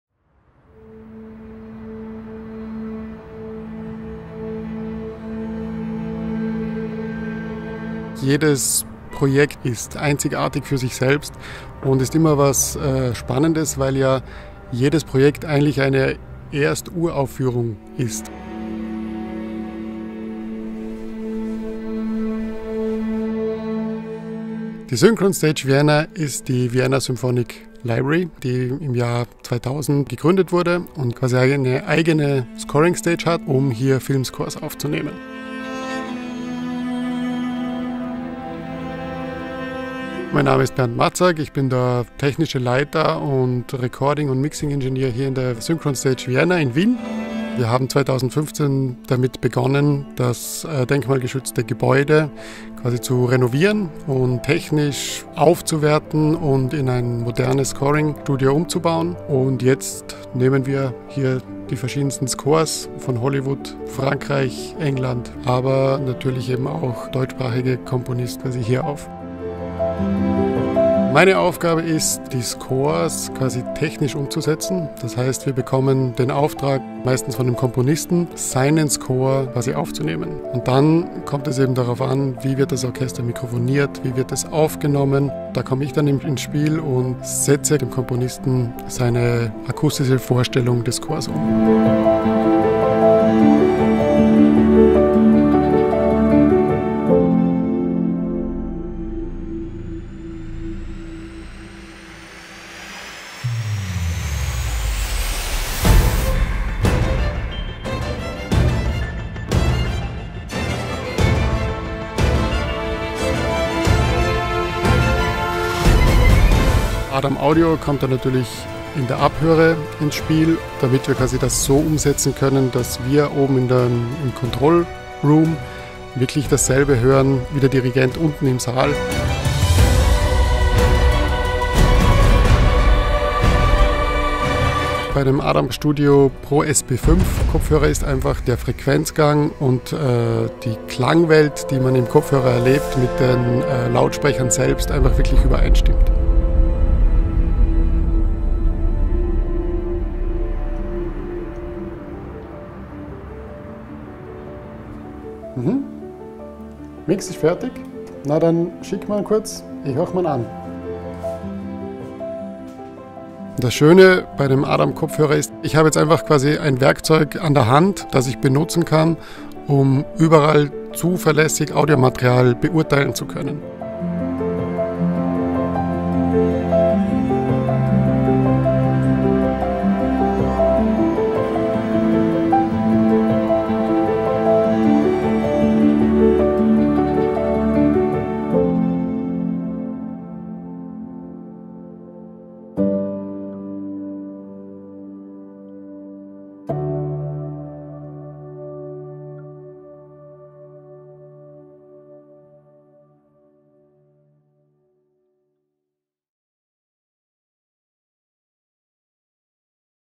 ADAM Audio - In The Studio With Vienna Symphonic Library at Synchron Stage Vienna
We had the pleasure to meet Bernd from VSL, to whom we had sent our new SP-5 Studio Pro headphones shortly before.

Of course, we were very interested in hearing his opinion about our new product!